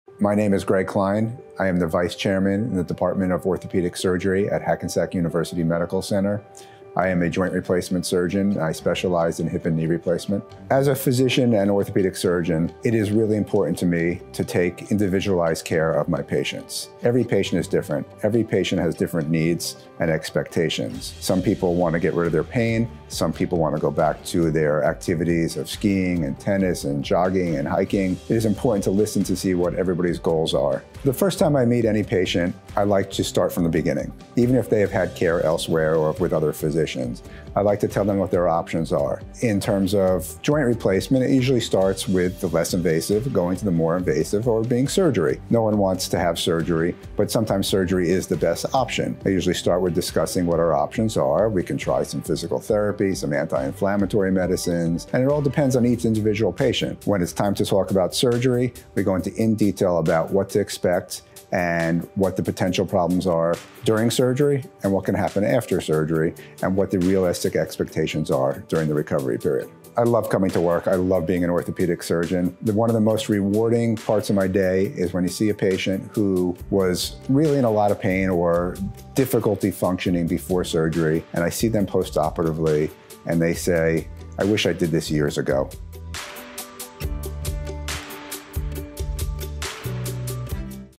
Gregg Roger Klein, M.D. - FAD Video
Gregg Klein, M.D., is a fellowship trained, board-certified orthopedic surgeon who specializes in primary and revision hip replacement, partial knee replacement, and primary and revision knee replacement. Dr. Klein and his colleagues recently established a new practice, Orthopedic Specialists of New Jersey.
Dr. Klein has particular interest in robotic joint replacement surgery, and is one of the developers and designers of a robotic knee replacement system used worldwide. He is an expert in specialized surgical techniques including direct anterior and mini-posterior hip replacement, and minimally invasive partial and total knee replacement. His specific areas of interest include hip and knee arthritis, avascular necrosis, osteonecrosis, and rheumatoid arthritis. Dr. Klein’s goal is to improve the quality of life for each patient by helping them resolve their symptoms and getting them back to doing the activities they love.

He has served on the editorial board for the Journal of Bone and Joint Surgery, Clinical Orthopedics and Related Research, and the Journal of Arthroplasty.  Dr. Klein has also published numerous scientific articles, textbooks, and book chapters in the field of orthopedics and joint replacement. He is actively engaged in ongoing joint replacement research. He is recognized as a Top Doctor by New Jersey Monthly, Castle Connolly, Bergen County Magazine and 201 Magazine.

Away from the office, Dr. Klein enjoys spending time with his wife and their two daughters. He is an avid runner and skier, and a frustrated golfer.